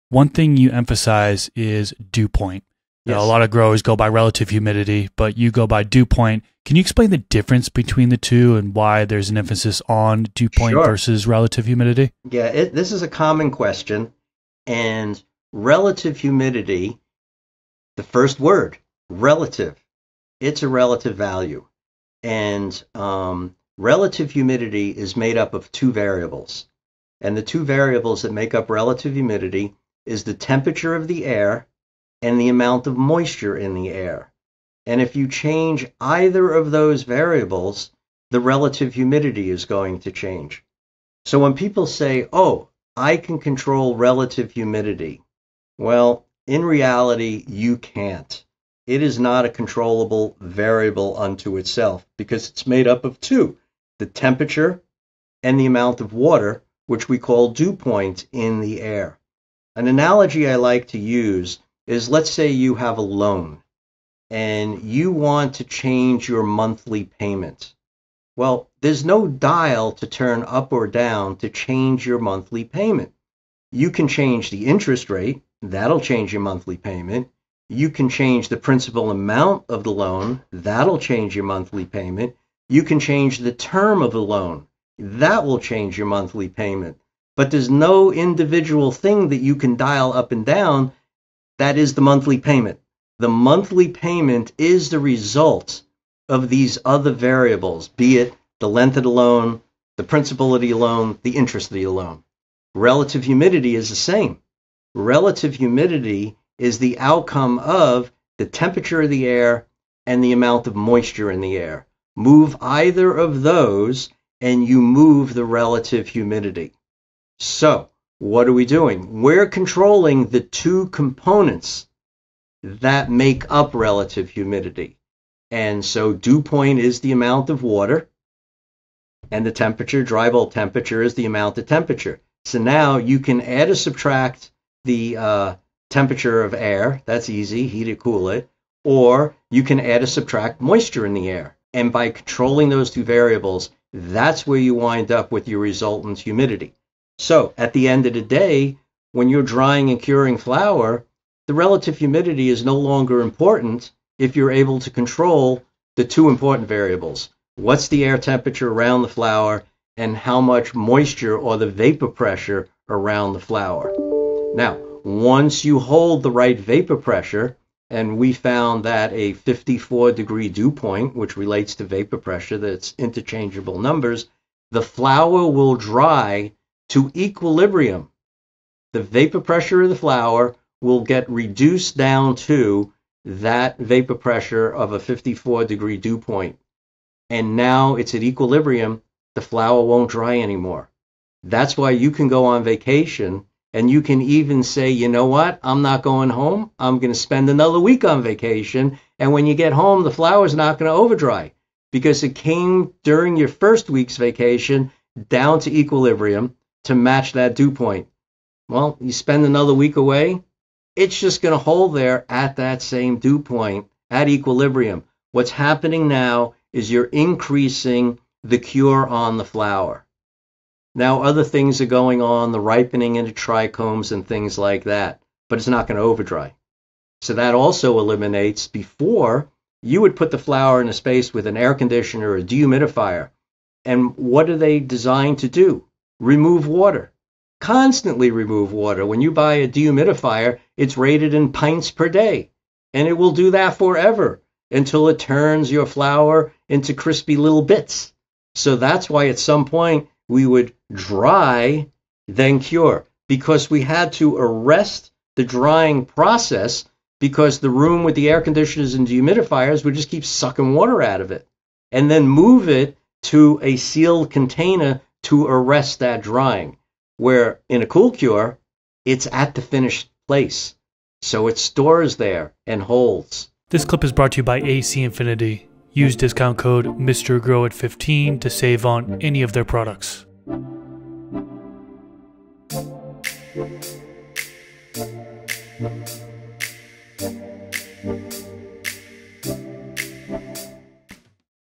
Using Dew Point vs Relative Humidity When Drying Plants!
In this Garden Talk clip, David Sandelman talks about drying plants and the difference between dew point and relative humidity. What are your thoughts on this?